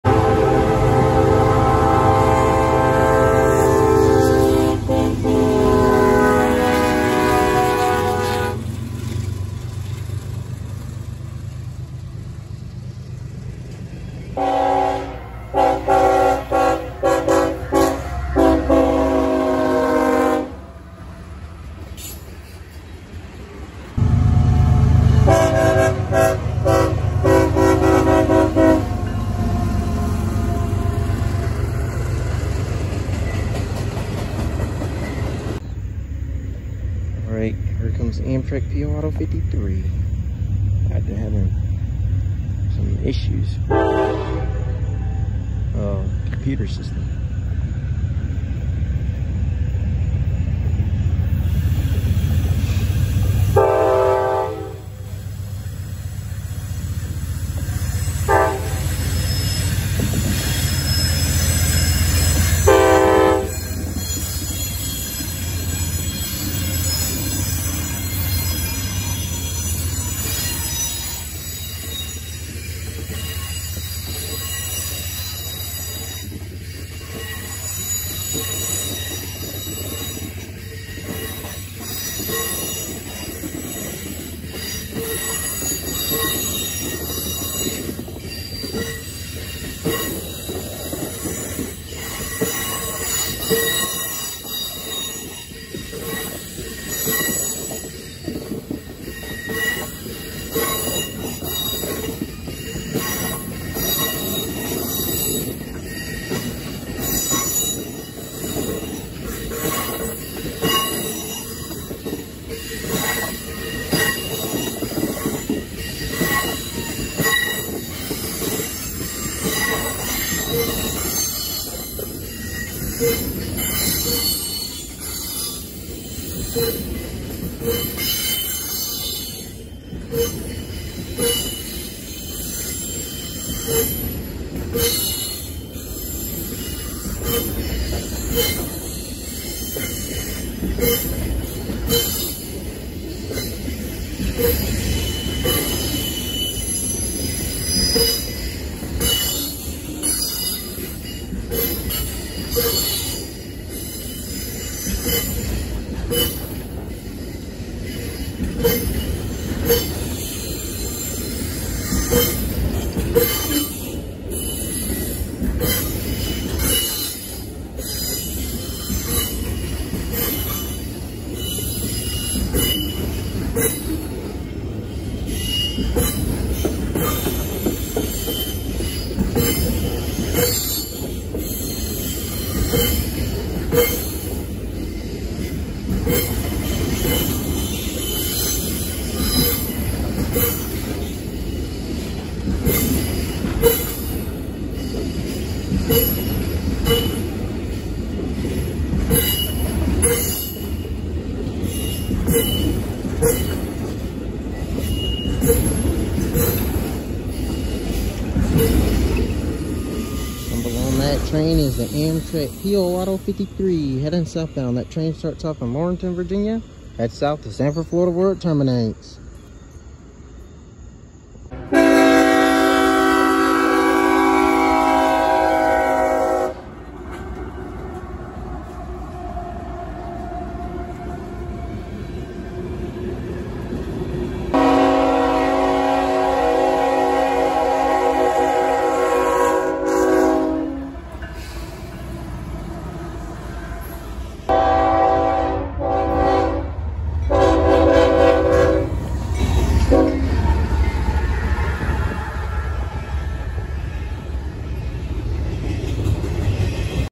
Amtrak P053 Auto Train SB Engines 835 & 830 Florence SC
Railfanning With @ATSRailPro